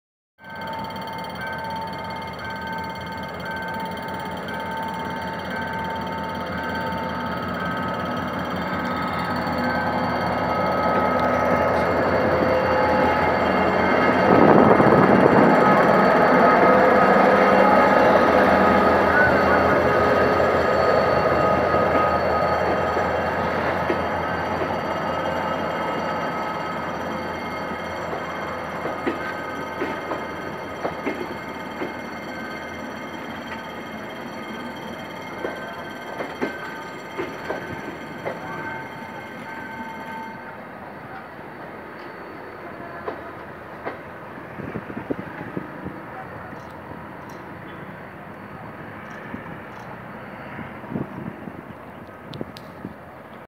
GM GT-22 CW-2 A916 con tren de pasajeros 4515 Temperley-Haedo de las 10 llegando a Haedo
El jueves 7 de julio de 2022 a las 11.15